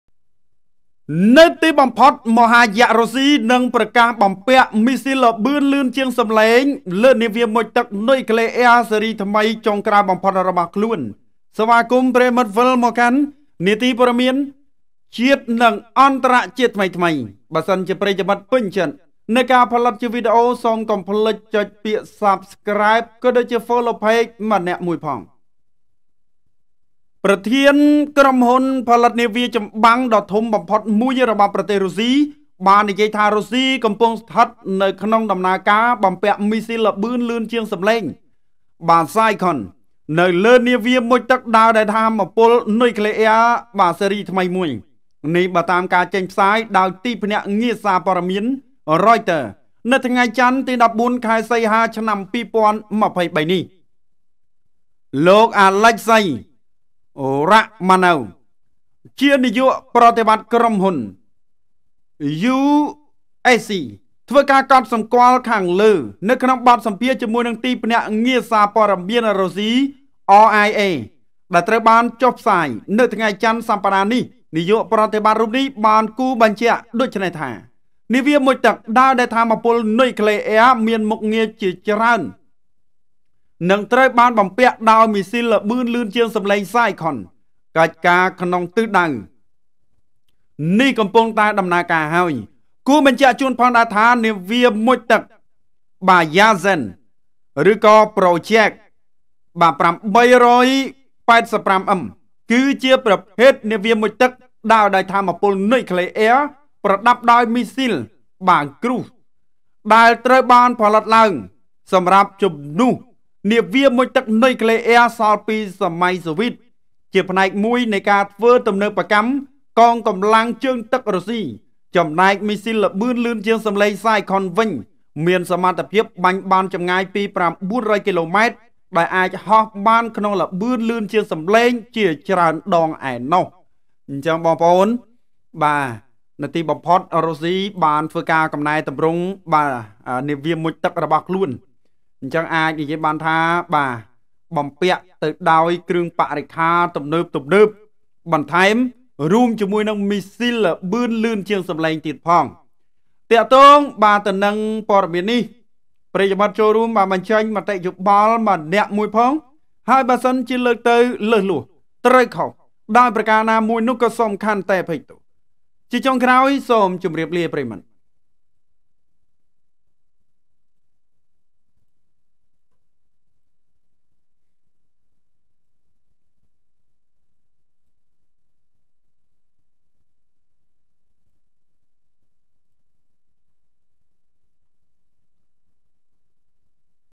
Russia to install supersonic missiles on new nuclear submarines
រុស្ស៊ី នឹងបំពាក់មីស៊ីលល្បឿនលឿនជាងសំឡេងលើនាវាមុជទឹកនុយក្លេអ៊ែរស៊េរីថ្មីត្រៀមកម្ទេច..Russia to install supersonic missiles on new nuclear submarines